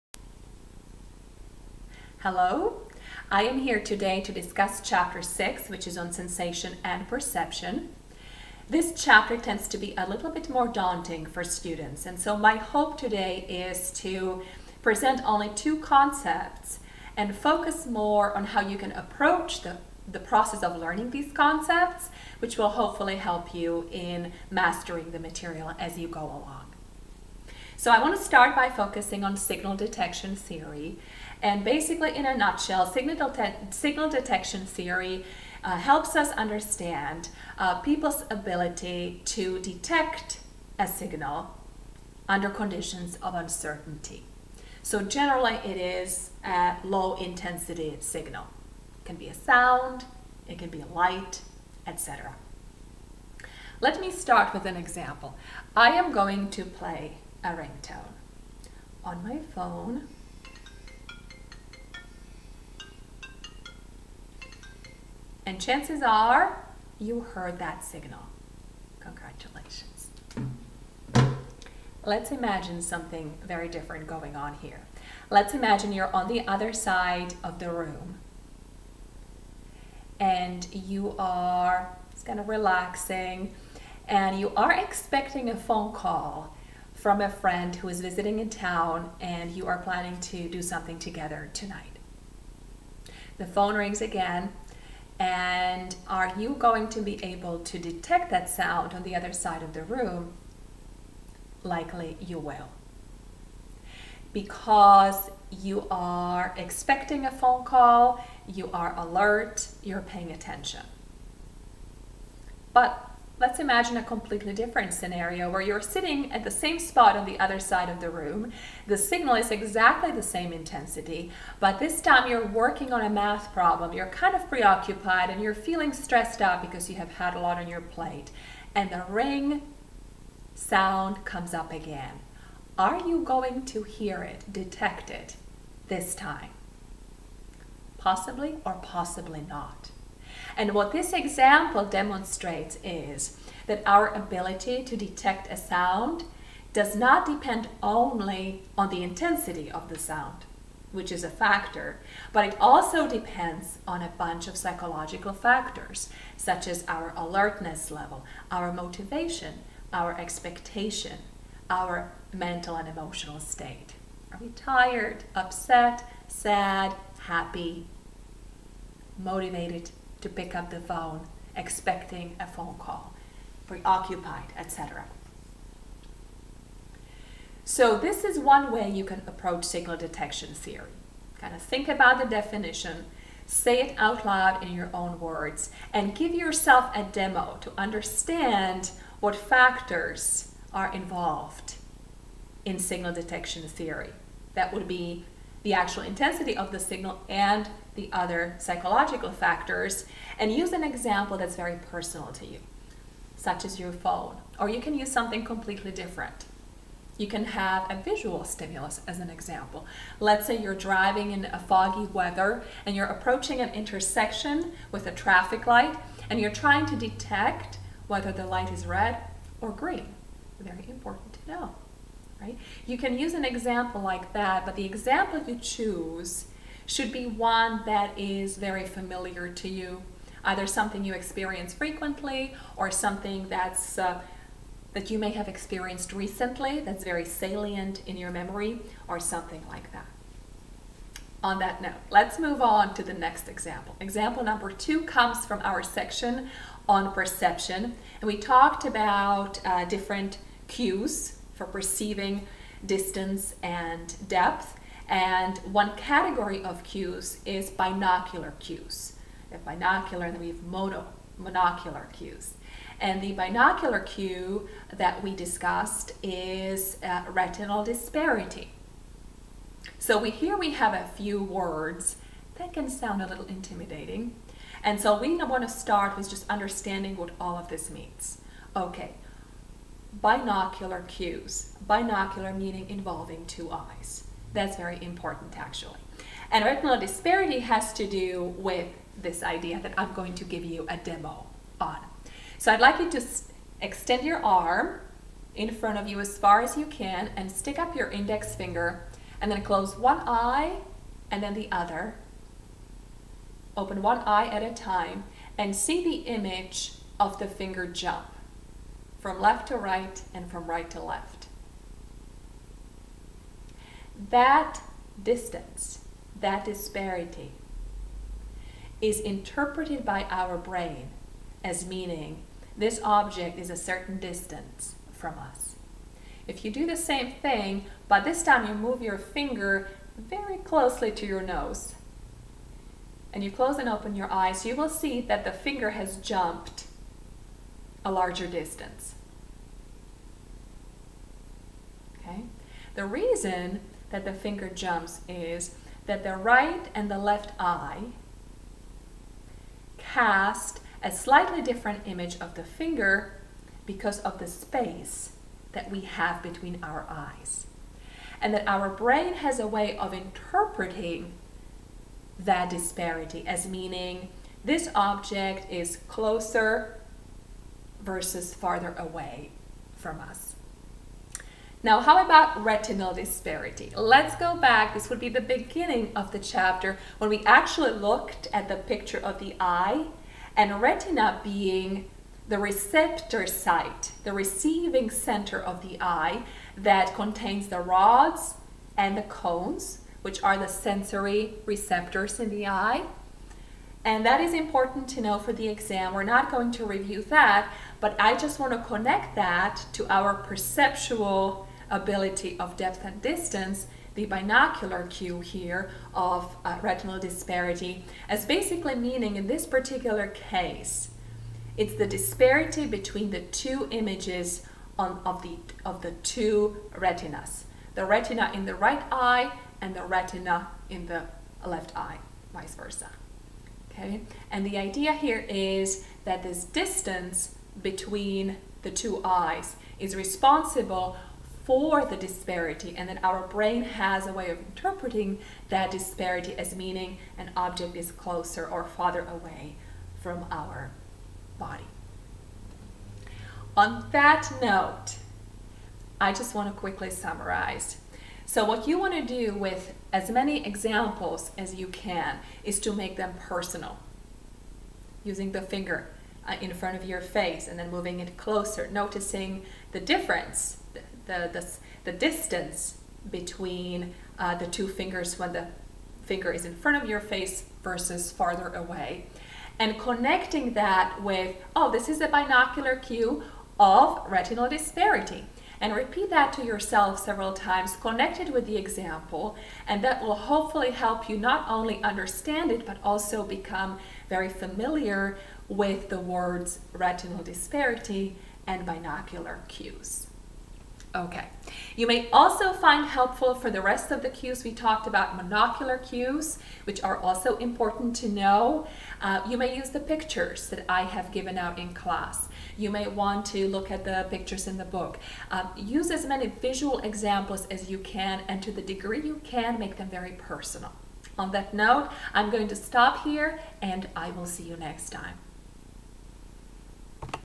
Chapter 6 Sensation and Perception (Intro to Psychology)
A brief review of two topics (Signal Detection Theory and the Binocular Cue of Retinal Disparity) with focus on learning strategies.